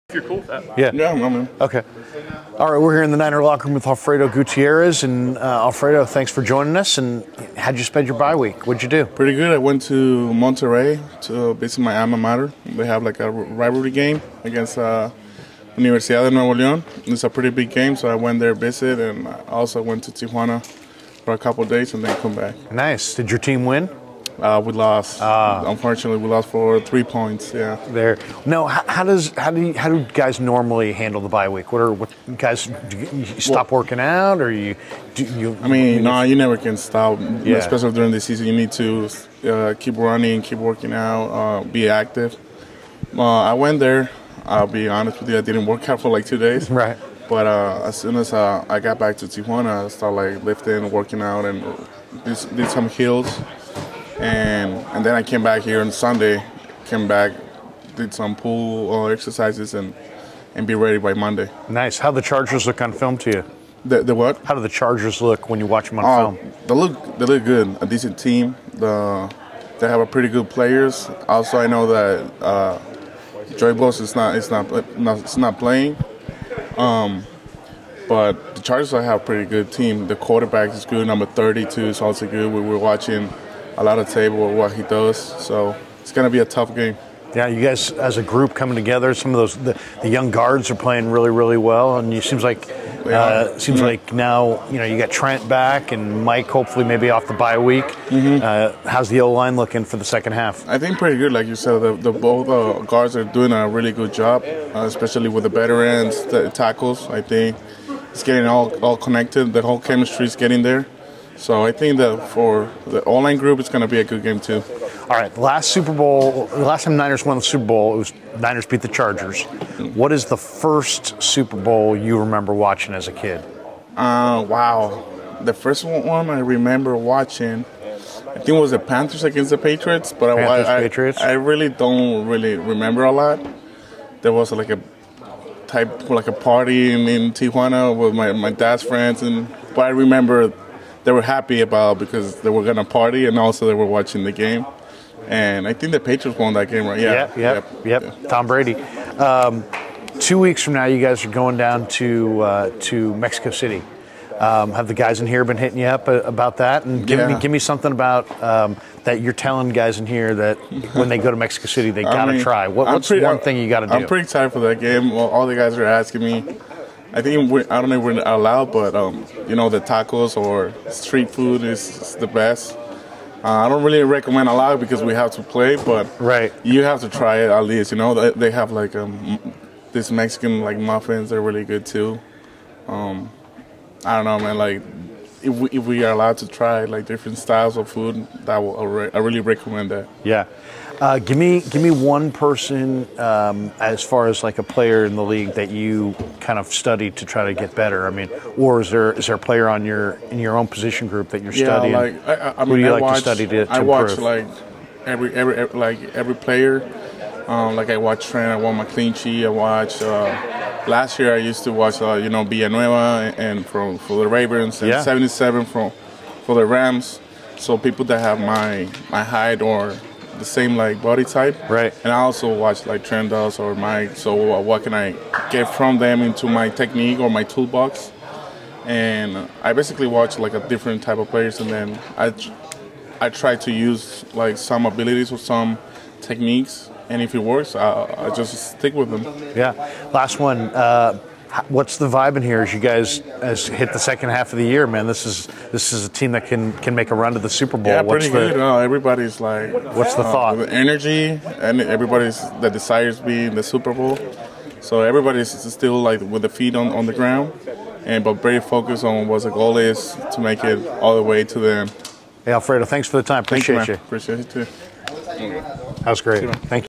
49ers Alfredo Gutierrez on upcoming game in Mexico City
Larry Krueger interviews  Alfredo Gutierrez who Grew up playing youth football in Mexico before moving to the U.S. Upon graduating high school, he enrolled in junior college before returning home to Mexico and joining Tec de Monterrey on a full athletic scholarship. Graduated from Tec de Monterrey, where he won a National Championship in 2019. Joins the 49ers as part of the NFL's International Player Pathway program, will be on the roster until the end of training camp, where he is eligible for an international player practice squad exemption, granting the 49ers an extra practice squad member that is ineligible to be activated during the 2021 season. Born 12/29/95 in Tijuana, Mexico.